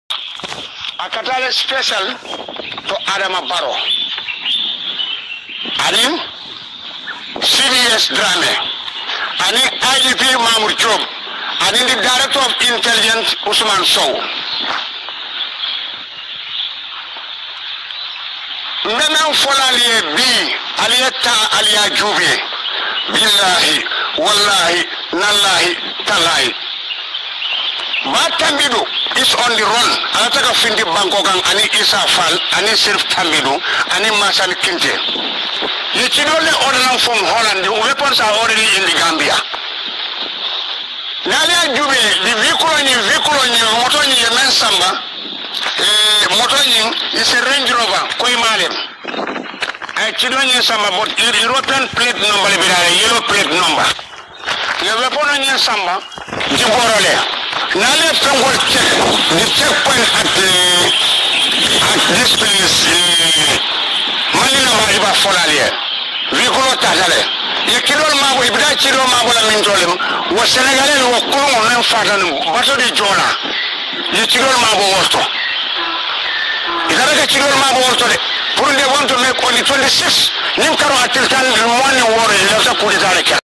Concern Gambians, this one is a serious matter, it comes to our notice that we have some bad element
Concern Gambians, this one is a serious matter, it comes to our notice that we have some bad elements wanting to destabilise our peaceful country. Masanneh Kinteh,Ba Tambadou, Sheriff Tambadou and Essa Faal are on the run to leave the country, President Adama Barrow and government should not let them leave the country, because they brought guns to overthrow the government, those guns are as follows, AK47=28, SNIPER GUN=8, RPG= 6, and MARK=4, this guns are from Holland and are in the at moment country, when they flew the country, their ground forces will be Wassa Camara, Lamin Sanyang and his deputy, Sp Jobe, and Abdoulie Manneh, the guns were transported by a Range Rover colour white with a European yellow number plate, they hide the guns in Giboro, their targeted date was on 26th June 2020 for their operations, we are making our security to know what is going and for the safety of their lives.